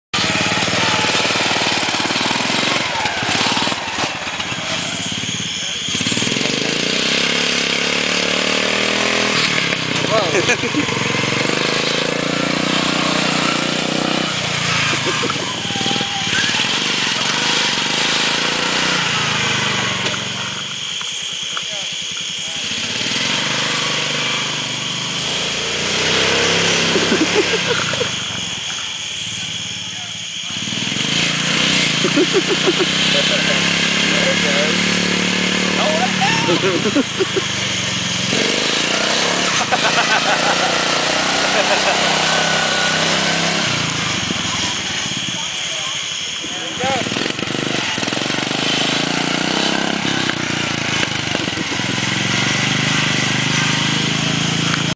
Night wheelies
Stupid tractor tricks by the bon fire.